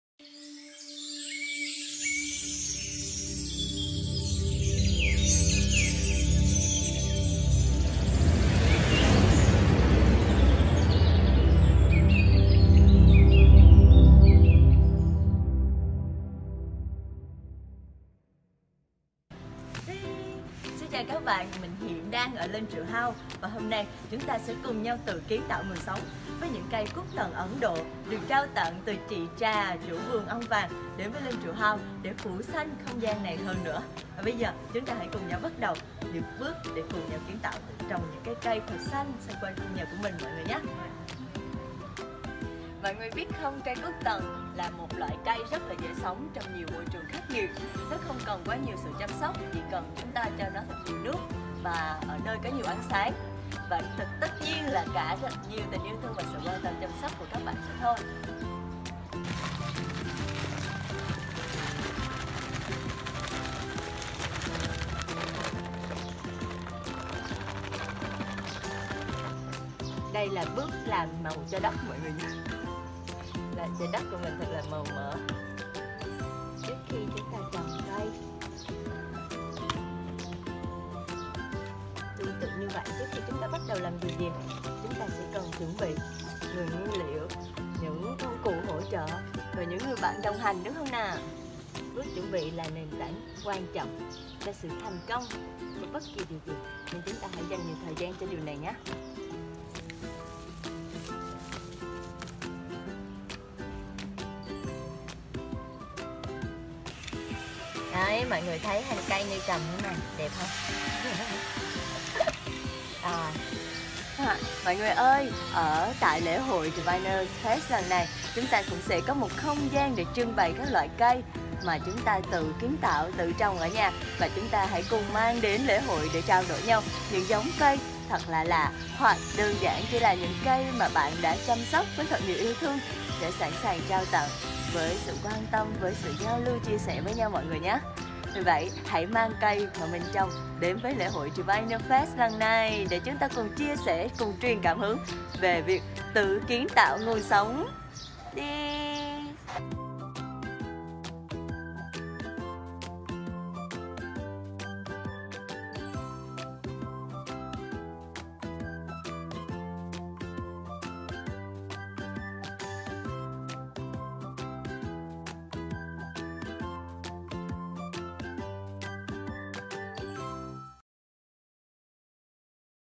Diviners Fest 2023 - Trồng Cây Phủ Xanh Tổ Ấm
🌱 Cùng đội ngũ Diviners Fest trồng cây phủ xanh tổ ấm.

Diviners Fest 2023 - Tự Kiến Tạo Nguồn Sống là một Lễ Hội Xanh, không rác thải nhựa, thuần chay, là sự kết hợp giữa Nghệ Thuật và Thiền Định.

Với tinh thần ai cũng là người kiến tạo, thân mời tất cả các bạn ai có cây có thể mang tới trưng bày và trao tặng cho nhau, thêm sắc xanh mát lành cho lễ hội.

🙏 DIVINERS FEST 2023
Vườn Ong Vàng, Hà Nội 19-20/11 💫